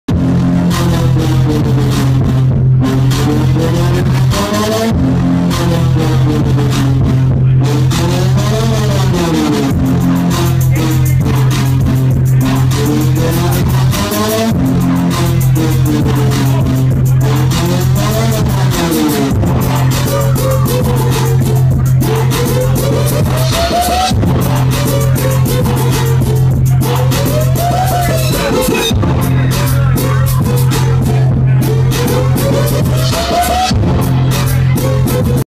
Sound Pieces presents Mimosa
Mimosa performing at Monarch in San Francisco, February 10, 2015. Visuals by WET